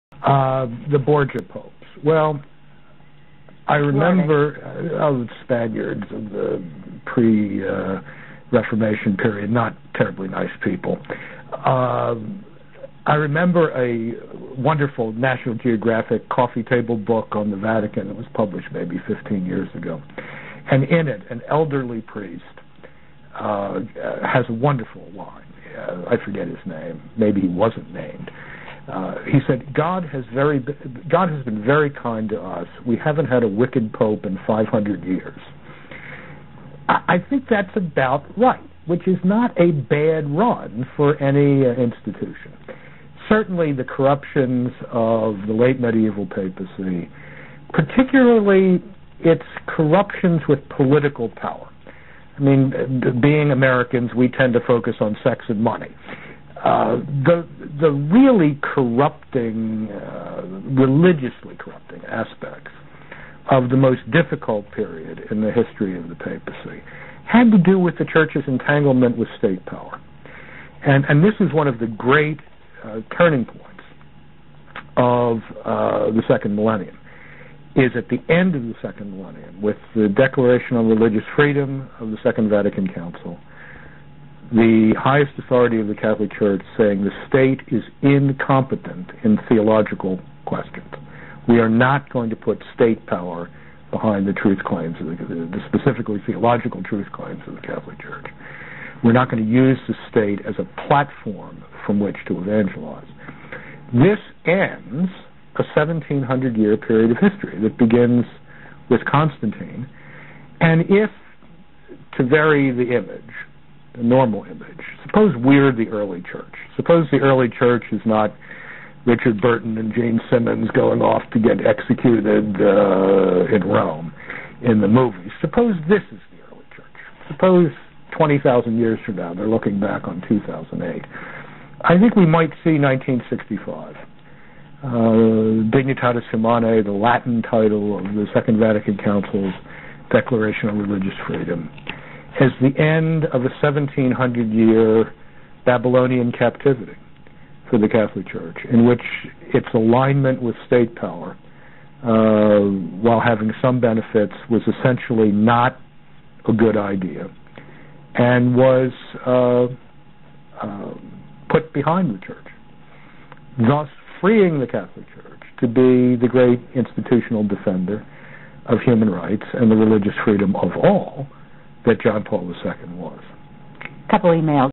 George Weigel on the Babylonian Captivity of the Church
George Weigel teaches us Vatican II set the Church free from the Babylonian Captivity that started with Constantine and continued with popes like Boniface VIII, Innocent III, and Pius IX. The papacy was a negative obstacle to the freedom movements in Europe (French Revolution, Italian Unification, Spanish Peoples War against Franco, etc) and Vatican II removed the threat of Church interference in state power, so that political actions can be guided by more capable hands. George Weigel correctly called this tumultuous 1700 year period of the Church: the Babylonian Captivity. Now the Church can turn her attention to more important matters: freedom for all humanity, including religious liberty - the freedom to believe in whatever God you choose.